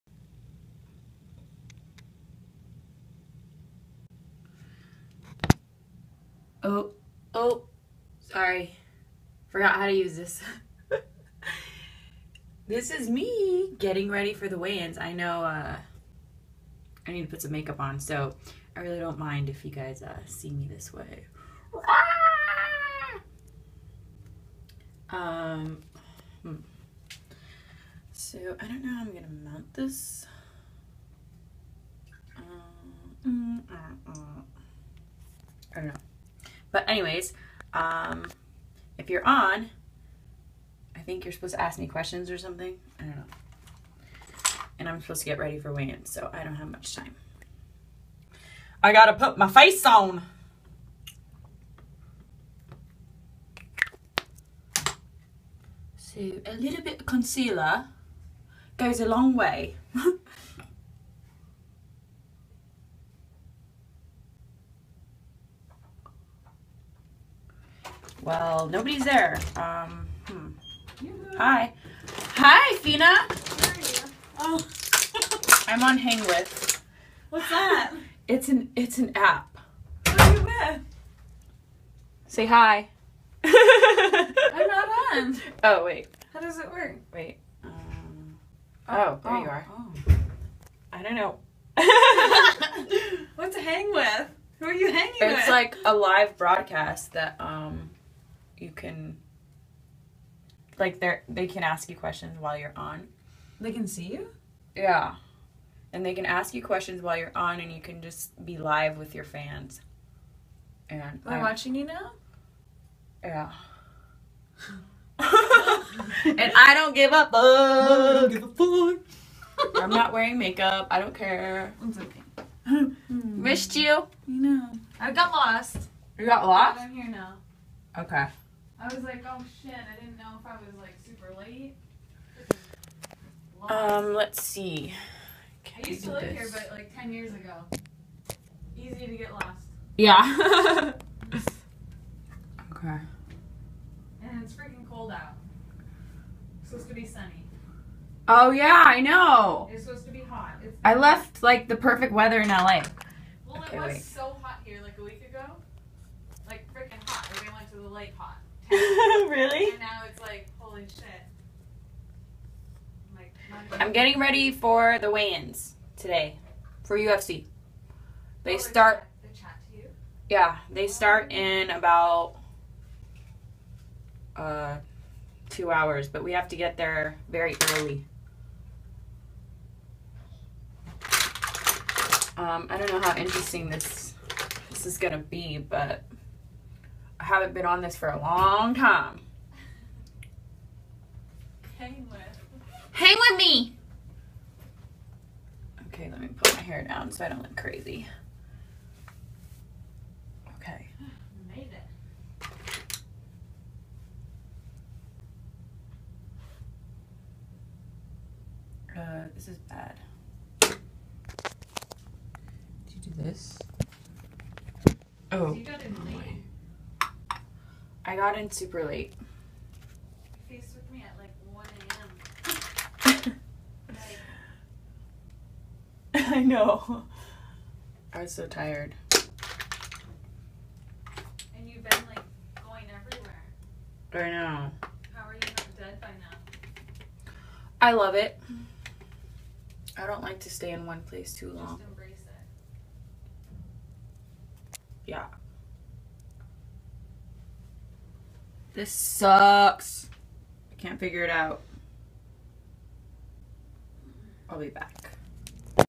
w/Arianny Celeste getting ready for UFC Weigh-ins
Hang w/Arianny Celeste in a live-stream broadcast.